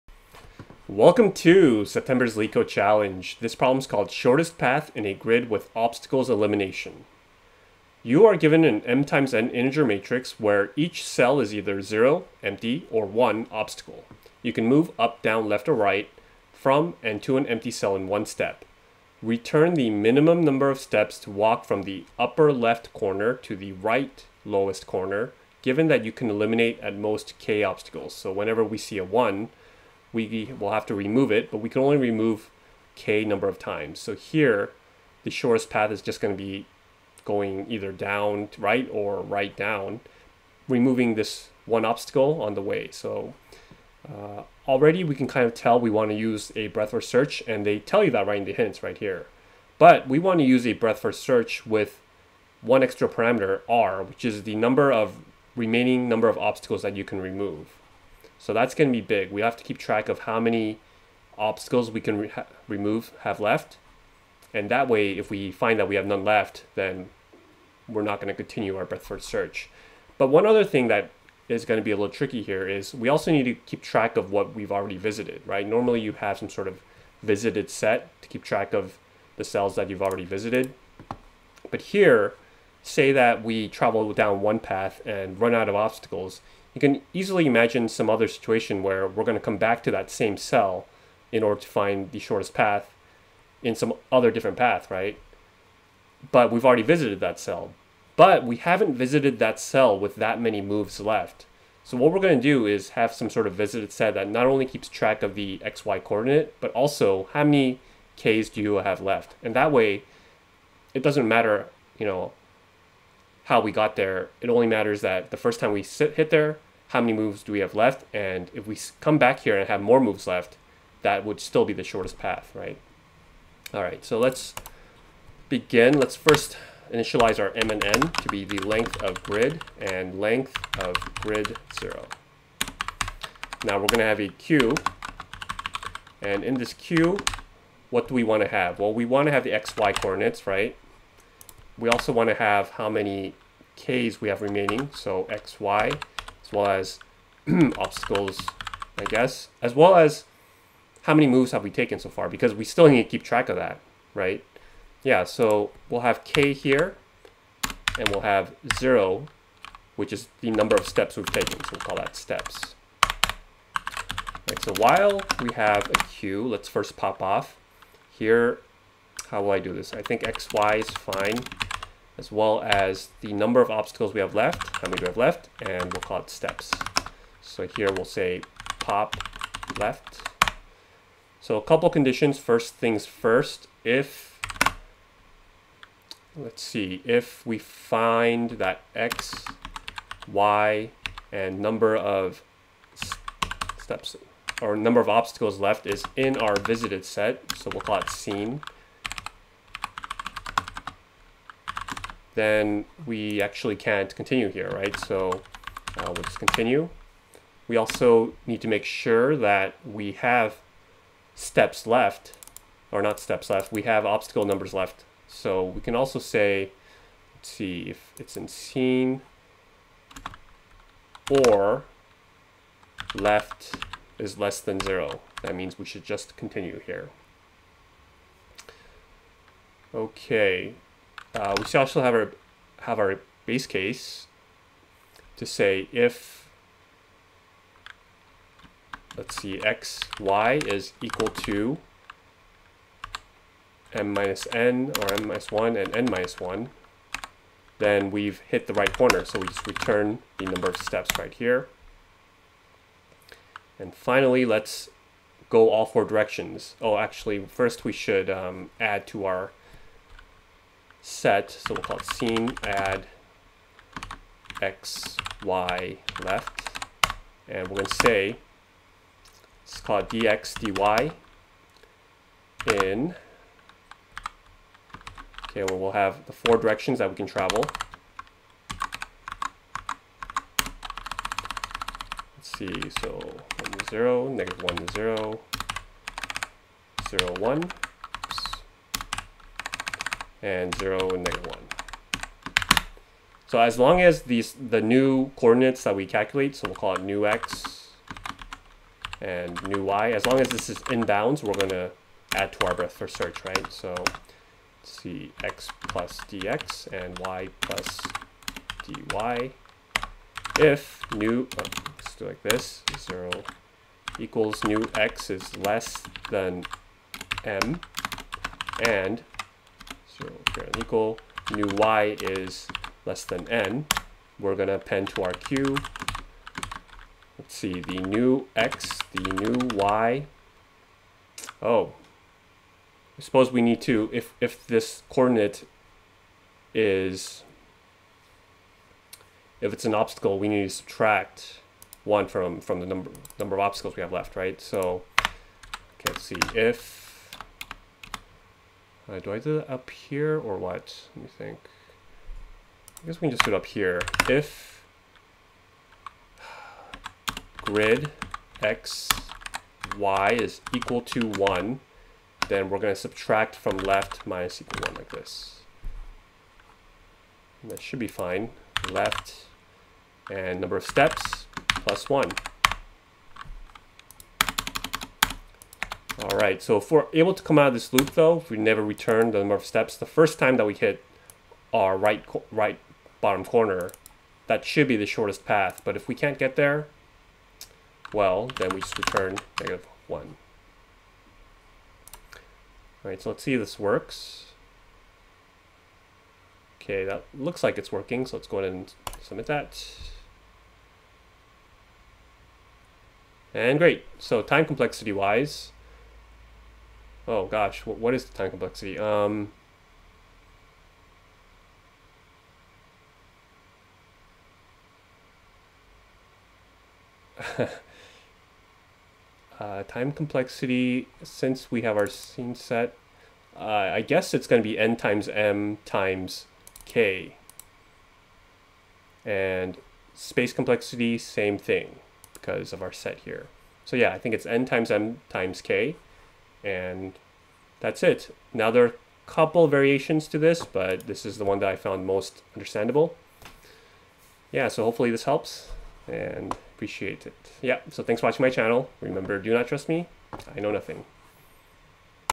Leetcode - Shortest Path in a Grid with Obstacles Elimination (Python)
September 2021 Leetcode Challenge
Leetcode - Shortest Path in a Grid with Obstacles Elimination #1293
Difficulty: Hard